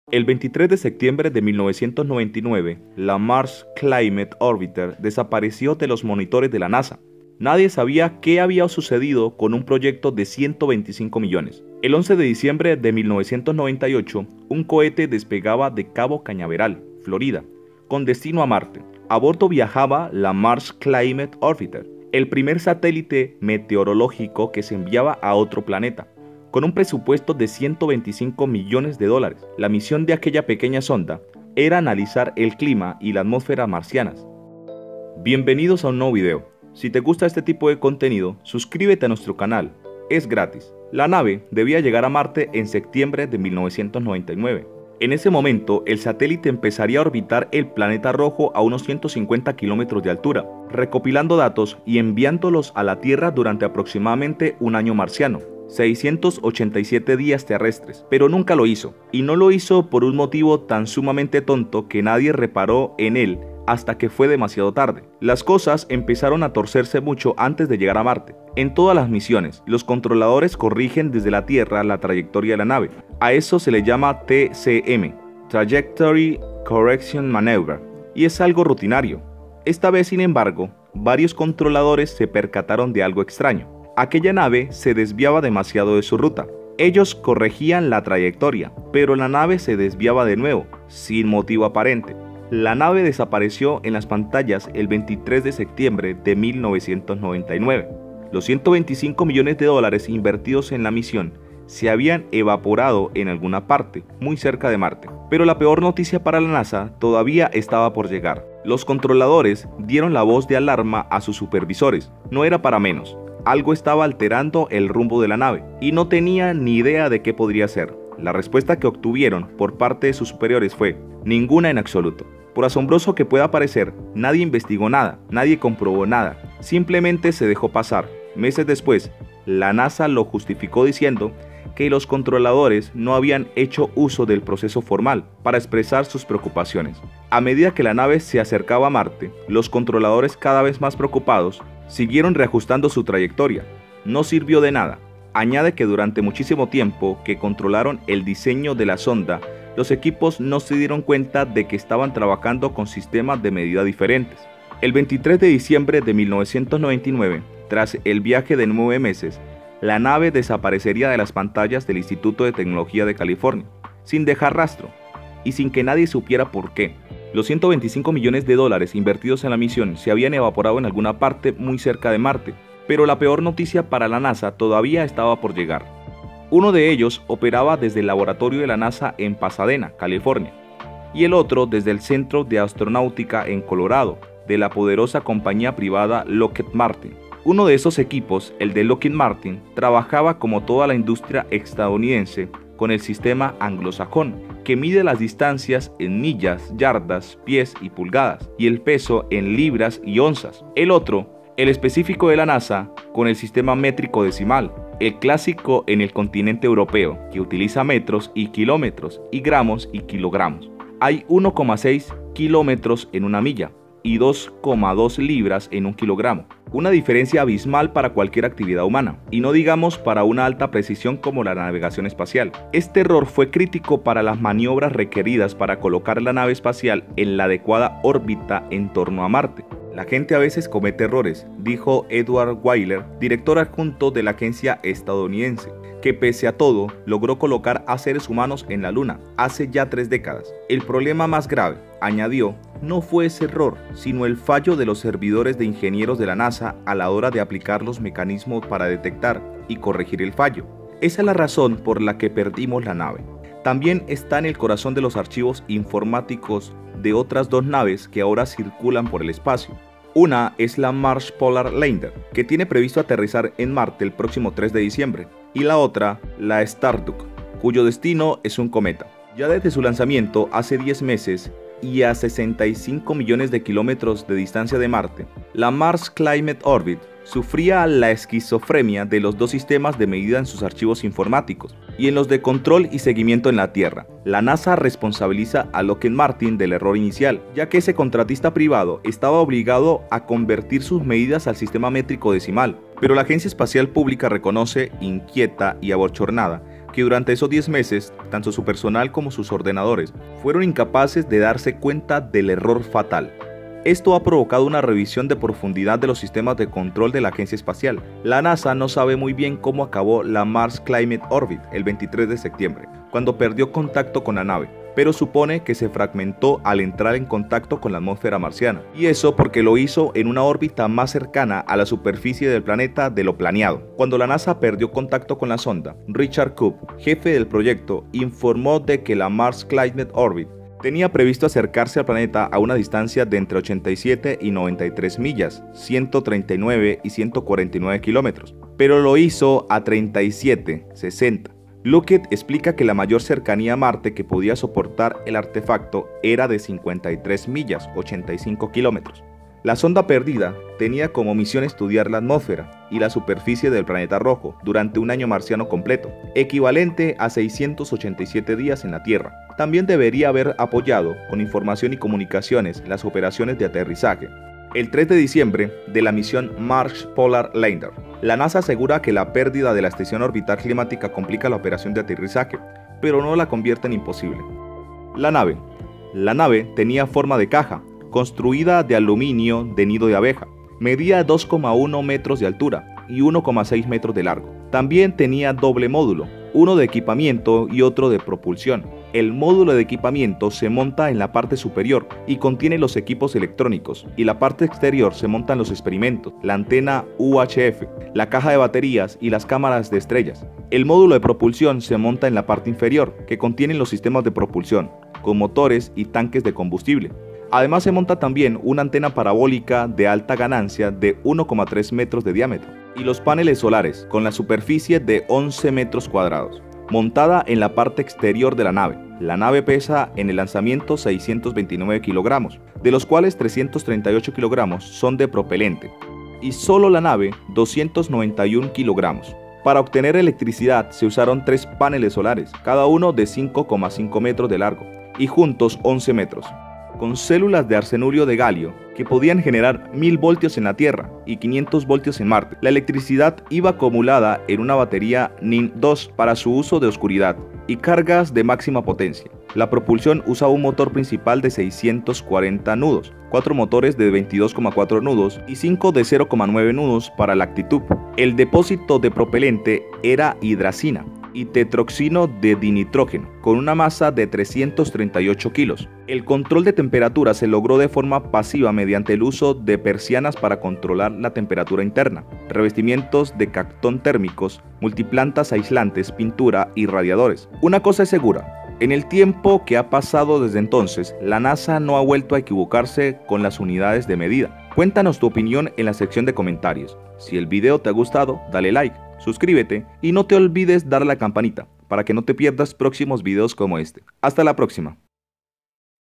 La NASA ha Tardado 35 Años en Darse Cuenta de que Había Cometido Este Enorme Error!
El 11 de diciembre de 1998, un cohete despegaba de Cabo Cañaveral, Florida, con destino a Marte. A bordo viajaba la Mars Climate Orbiter, el primer satélite meteorológico que se enviaba a otro planeta. Con un presupuesto de 125 millones de dólares, la misión de aquella pequeña sonda era analizar el clima y la atmósfera marcianas.